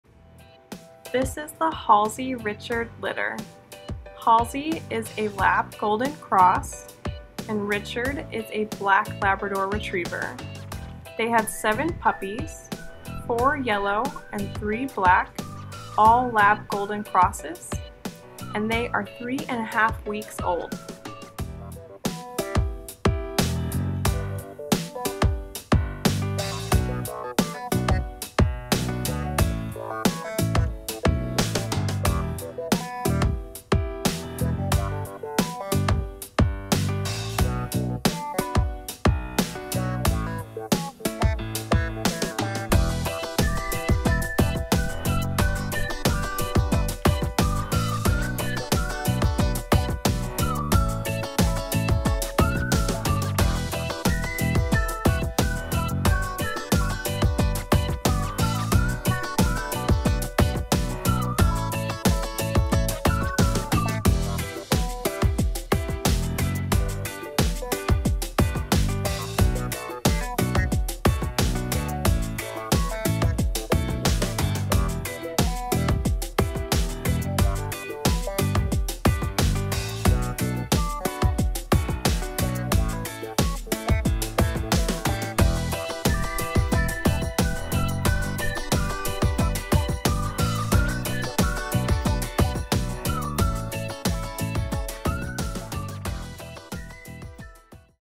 10-15-2020 Halsey
Halsey x Richard litter. 4 Yellow and 3 Black Labrador/Golden Retriever Cross puppies, 3.5 weeks old, DOB 09-21-20.